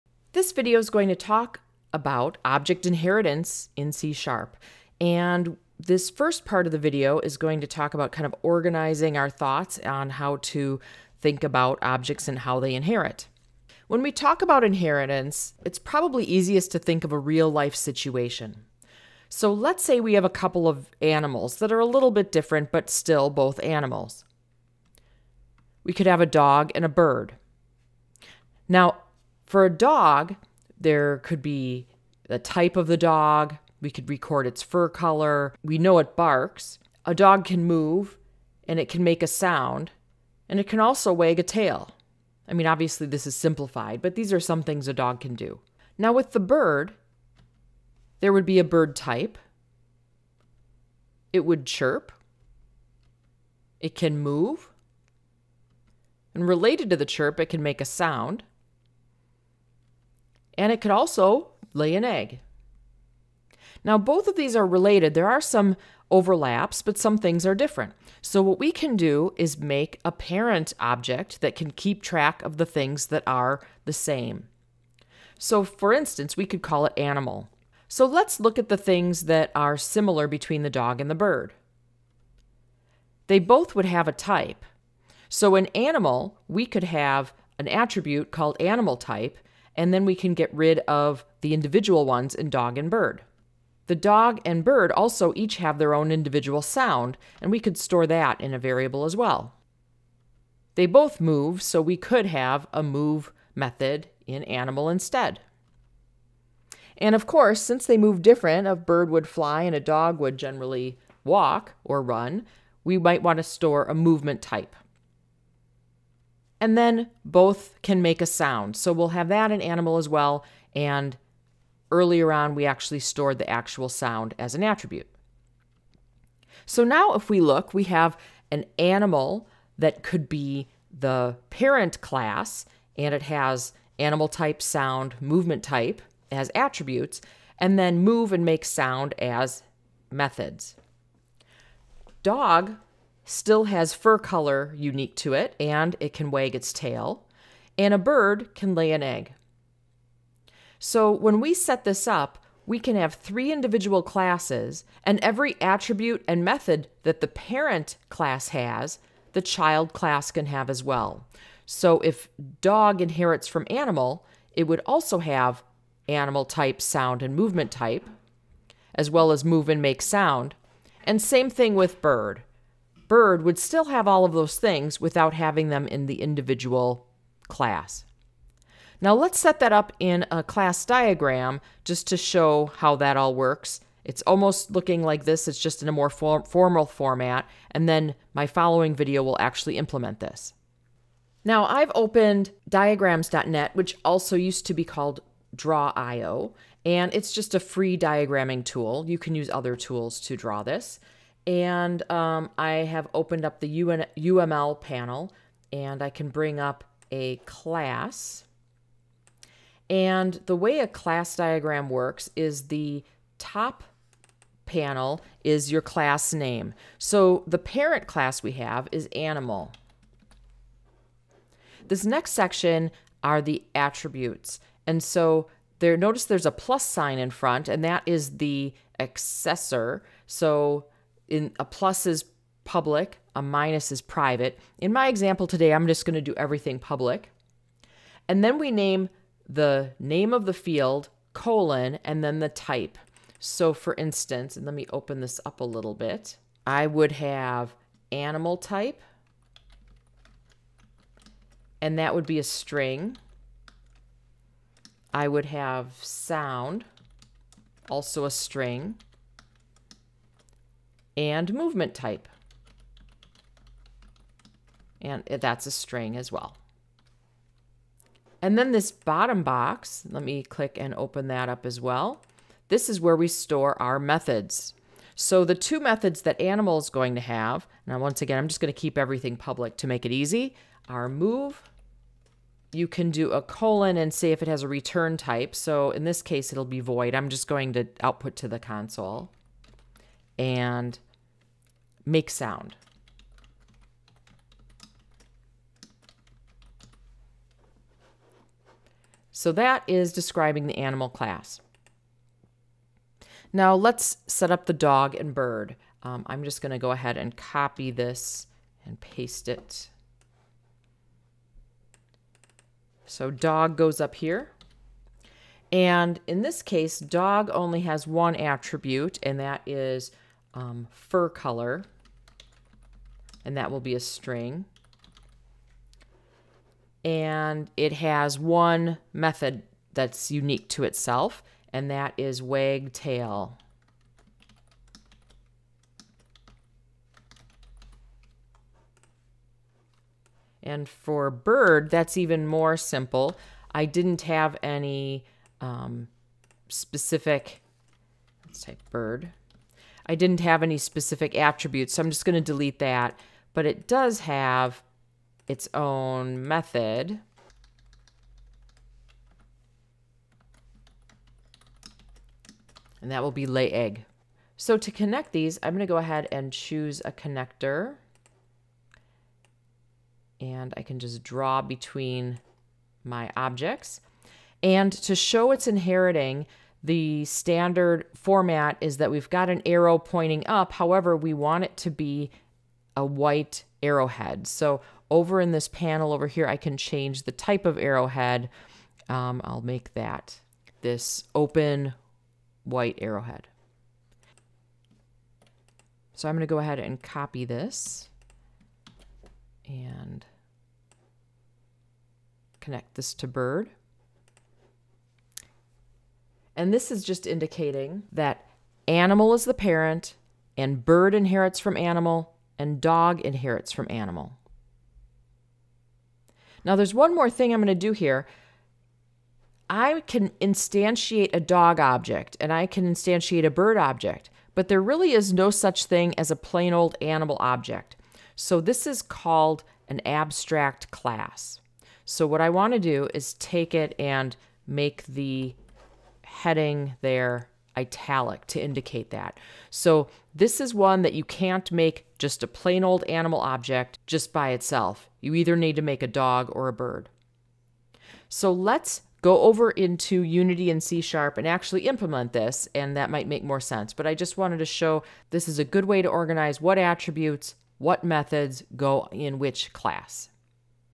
Basic Object Inheritance with C# and Unity, Part 1: Intro and Class Diagram
Beginner level overview of what inheritance is and how to organize the information into a class diagram.

This video is the first of 3 parts - here are links to the other 2: